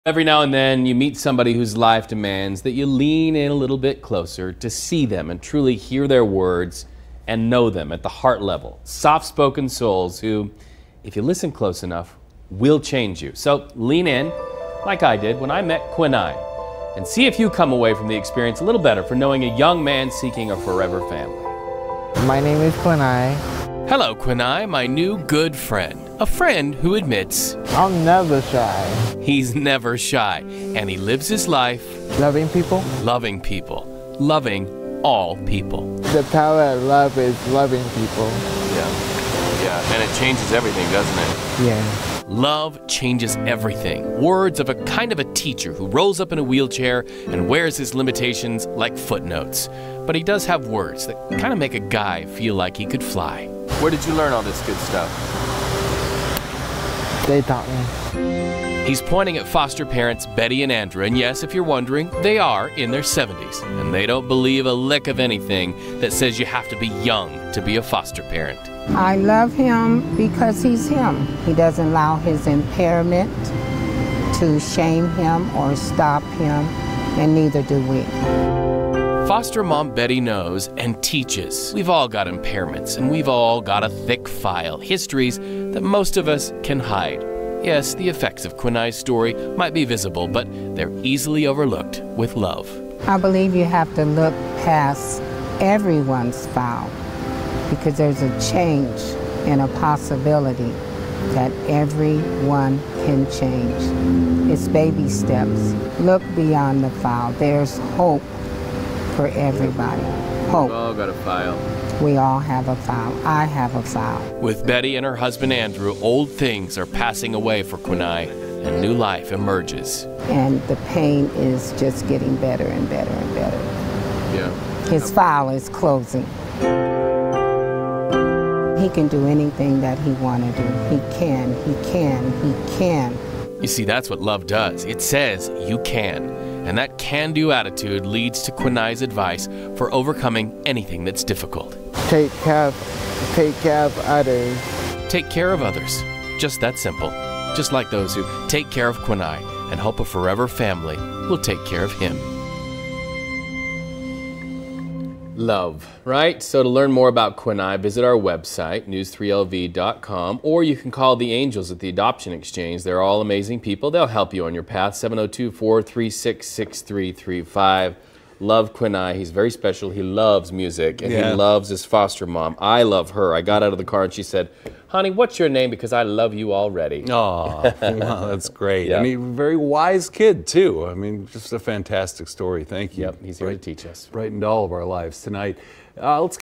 WEDNESDAY'S CHILD: Quineye believes in the power of love, and hopes a family will love him
Every now and then, you meet someone whose life demands you lean in a little to see them and truly hear their words to know them.
These people are soft-spoken souls who, if you listen close enough, will change you.